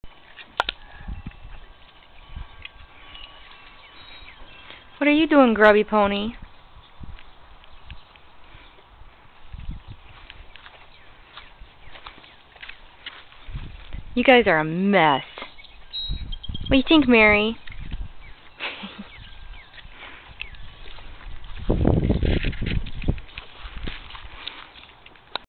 Sparrow and Mary
Munching their breakfast, getting a sip, and generally looking grubby because I haven't groomed them yet today and they are starting to shed and they have been rolling in the dirt.